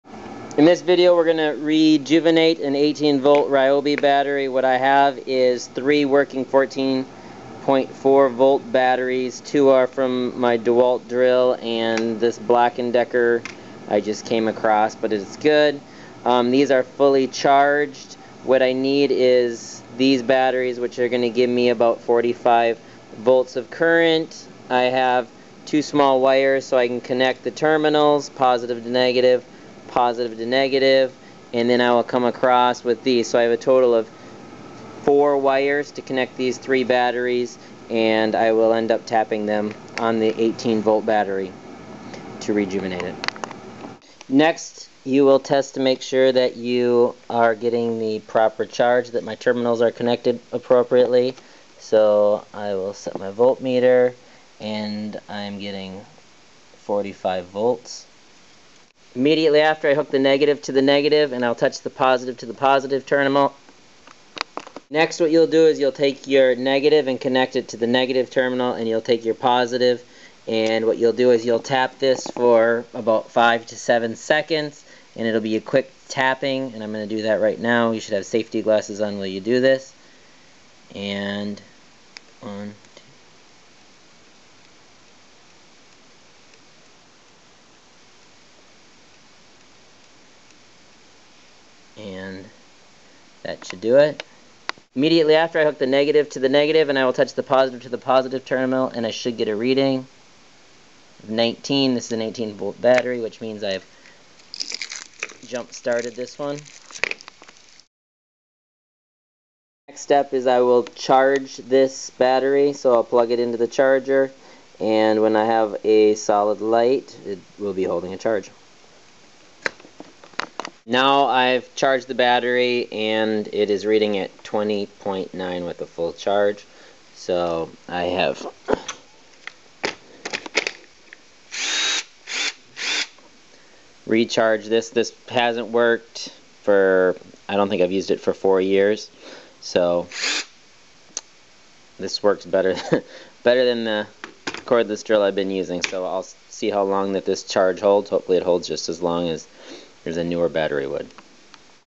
DIY How to revive/fix/rejuvenate/Repair/ recharge a bad dead rechargeable NiCd battery for cordless
Only works on NiCd batteries.  Do not try on other battery types.

In this video I take an old 18V NiCd battery that has not been used for 4 years, because it wouldn't hold a charge.  My Ryobi battery that I used on all my cordless tools (circular saw, drill, reciprocating saw, vacuum, rotary saw, sander, and flashligh).  You will need an alternate source that is around twice the voltage of the battery you want to rejuvenate.  I need 36 volts and was only able to combine the 3 batteries I had to make 45V.  I could have drained some charge down, but I decided to just use the 45V and it did the job. I connected my batteries in a series, but you can use any source that is about 36v.

It is important that you follow these steps to get proper results.  If you are not able to recharge after this you most likely have a dead cell.
1st  -  Fully drain your battery, so it shows no voltage on the meter.
2nd - Connect your batteries in a series.
3rd - Take the negative lead of your series and attach it to the negative terminal of your dead battery (I just taped it into place)
4th - take the positive lead from the series and quickly and repetitively tap it to the positive terminal of the dead battery for 5-7 seconds.
5th - Check current with the multimeter to make sure your dead battery accepted the charge.  If it did not wait 5-10 minutes and try again, before determining you have another problem, just in case you made a mistake in the process the first time.
6th - wait 10-15 minutes before attempting to charge.
7th - Fully charge battery and test test on multimeter.
8th - Enjoy your newly rejuvenated battery

This will work on most all NiCd batteries.  This is a must for anyone with a cordless drill.  Save your drill for years!

German
DIY Wie / fix wieder zu beleben / verjüngen / Reparatur / tanken einen schlechten toten wiederaufladbare NiCd-Akku für schnurlose
Russian
Как оживить / исправление / омолодить / Ремонт / перезарядки плохой мертвой аккумуляторную NiCd аккумулятор для беспроводного
Rabotayet tol'ko na nikel'-kadmiyevykh akkumulyatorov. Ne pytaytes' na drugikh tipov batarey.
Spanish
Cómo revivir / fix / rejuvenecer / Reparación / recargar una batería recargable muertos mal NiCd para inalámbrico

Sólo funciona con baterías de NiCd. No trate de otros tipos de baterías.

En este video me tomo una vieja batería de 18V NiCd que no ha sido utilizado desde hace 4 años, ya que no retiene la carga. Mi batería Ryobi que he usado en todos mis herramientas sin cable (sierra circular, taladro, sierra de sable, de vacío, sierra giratoria, lijadora, y flashligh). Usted necesitará una fuente alternativa que es alrededor de dos veces el voltaje de la batería que usted quiere rejuvenecer. Necesito 36 voltios y sólo era capaz de combinar las 3 baterías que tenía que hacer 45V. Yo podría haber drenado un poco de carga hacia abajo, pero decidí simplemente usar el 45V y no el trabajo. Me conecté mis pilas en una serie, pero se puede usar cualquier fuente que está a punto de 36v.

Es importante que siga estos pasos para obtener resultados adecuados. Si usted no es capaz de recargar después de esto es muy probable que tenga una célula muerta.
Primero - drenar completamente la batería, por lo que no muestra el voltaje en el medidor.
2 º - Conecte las baterías en una serie.
3 º - Tome el cable negativo de la serie y adjuntarlo a la terminal negativa de la batería muerta (Acabo grabó en su lugar)
Cuarto - tome el cable positivo de la serie y de forma rápida y repetitivamente aprovechar al terminal positivo de la batería descargada durante 5-7 segundos.
5 º - Comprobar actual con el multímetro para asegurarse de que su batería muerta aceptó el cargo. Si no se hizo esperar 5-10 minutos e inténtelo de nuevo, antes de determinar que tiene otro problema, en caso de que cometió un error en el proceso de la primera vez.
6 º - esperar 10-15 minutos antes de intentar cargarla.
7 º - Cargue completamente la batería y test test de multímetro.
8 º - Disfrute de su batería recién rejuvenecido

Esto funciona en la mayoría de todas las baterías de NiCd. Esto es una necesidad para cualquier persona con un taladro inalámbrico. Guarde su taladro por años! cualquier otra tipo de taladro con una batería de NiCd. Trabajos sobre 9V, 9.6V, 12V, 14V, 18V 14.4V, 19.2V, 20V, o 9, 9.6, 12, 14, 14.4, 18, 19.2, batería de 20 voltios hágalo usted mismo y traerlo de vuelta a la vida.